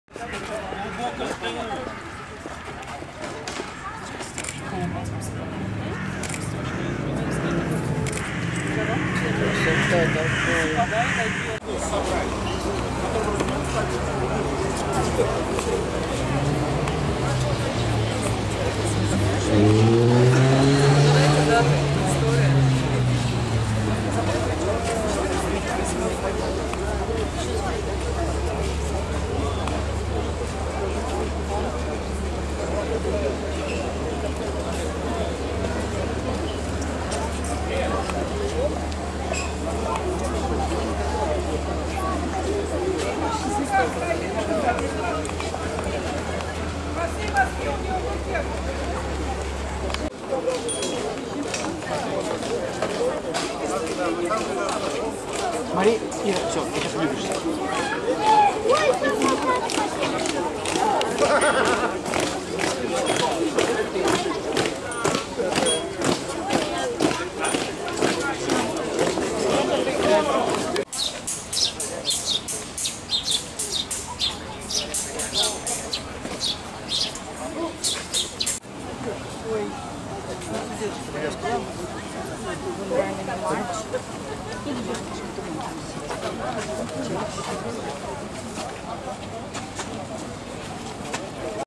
Времена и эпохи Собрание Посетители фестиваля
На все мероприятия фестиваля приходит очень много посетителей.